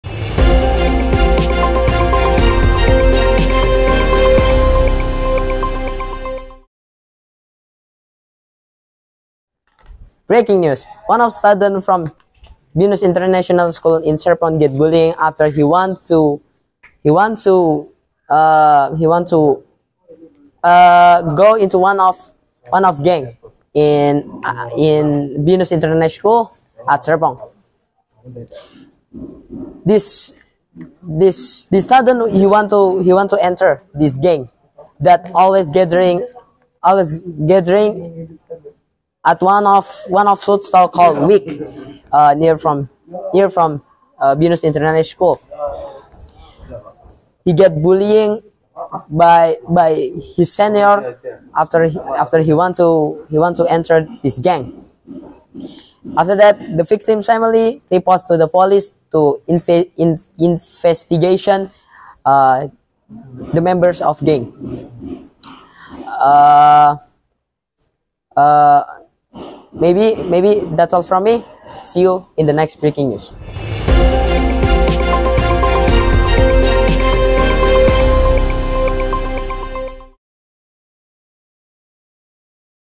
Bullying of classmates against younger classmates - Breaking News #4
Student Bullying at Binus International School, Serpong, Indonesia